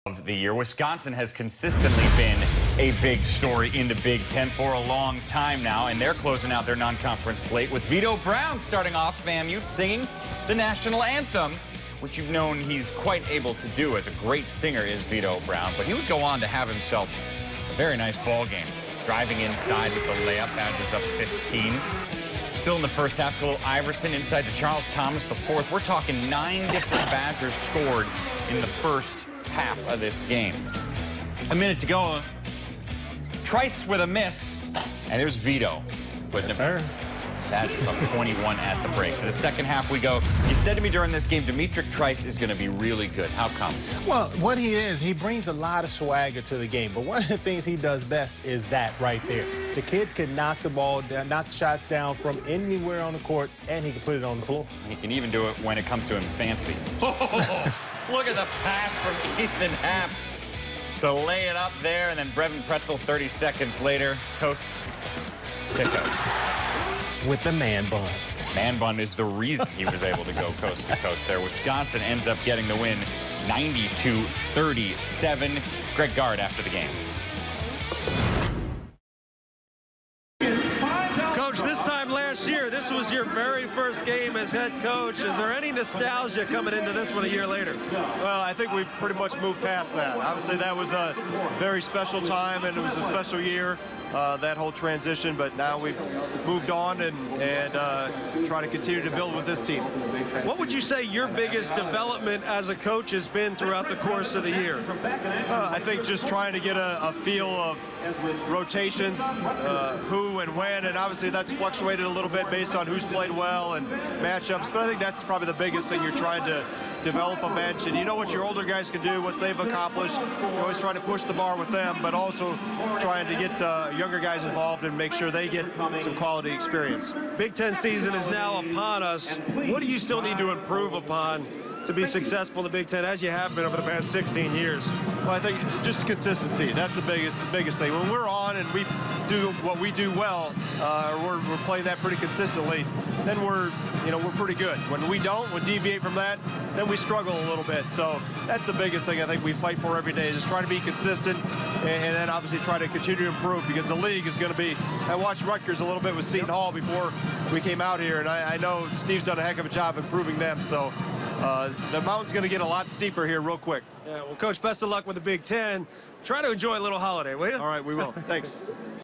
Florida A&M at Wisconsin - Men's Basketball Highlights
D'Mitrik Trice scored 14 points to help lead Wisconsin to a 90-37 win over Florida A&M.